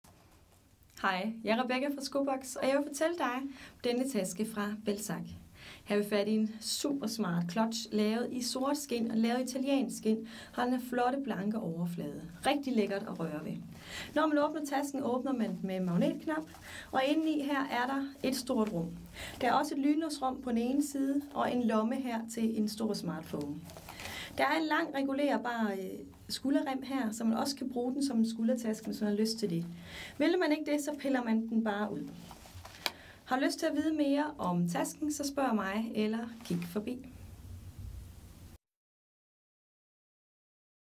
Skobox - Smart Belsac 13401 clutch i lækkert skind - Køb Belsac Tasker online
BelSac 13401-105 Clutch
Flot og elegant sort clutch fra BelSac i lækkert italiensk skind. Tasken åbnes med magnetknap og indeni er der et stort rum, lynlåsrum i den ene side og lomme i den anden side. Der medfølger lang skindrem, som kan tages af og på, så man også kan bruge tasken som skuldertaske.
H: 15 cm x B: 23 cm

Mærke:    BelSac
Køn:         Tasker & Accessories
Kategori:  Tasker
Type:         Clutch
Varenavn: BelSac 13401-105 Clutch